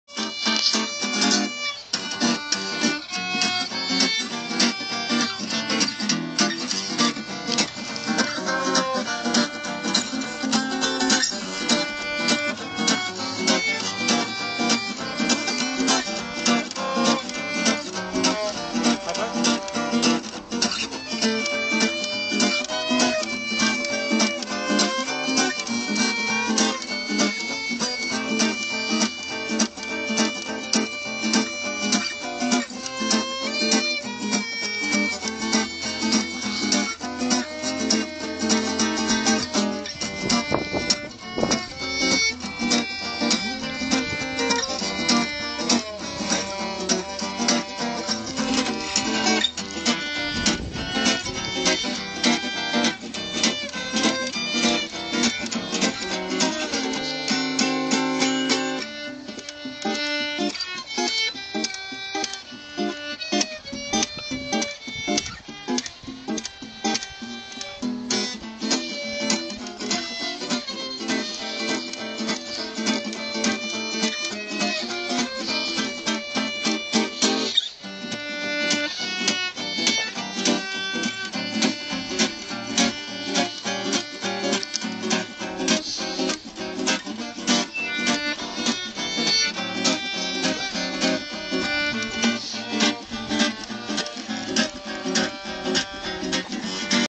Jennie and Ian
This is my two cousins, who are also brother and sister, improvising in the dark cemetary. He is 28 and playing guitar; she is 13 and playing violin.